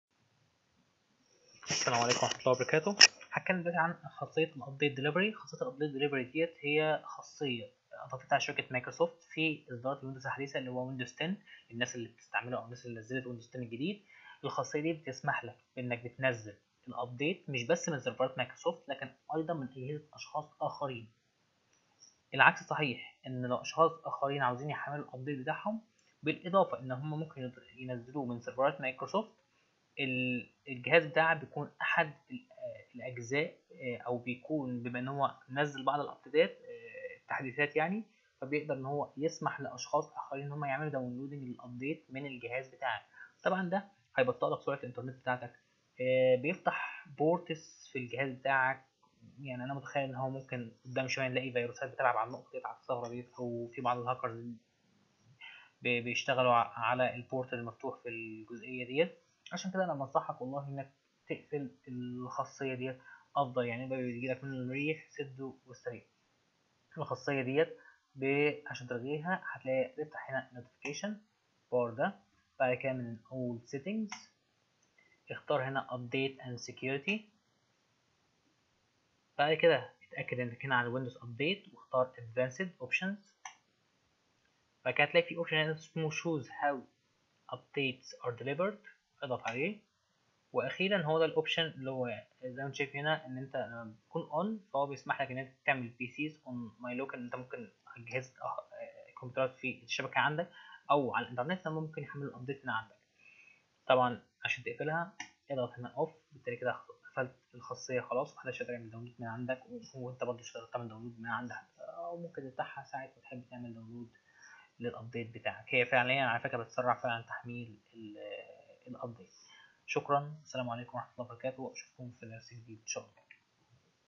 كيف تعالج مشكله بطئ الانترنت فى ويندوز 10
يقوم ويندوز 10 بإستخدام الانترنت لديك ليرسل التحديثات لاشخاص اخرين عن طريق جهاز و دون علمك  مما يؤثر سلبا على سرعة الانترنت لديك.
إليكم حل هذه المشكله و كيفية ايقاف خاصية ارسال التحديثات إلى اخرين.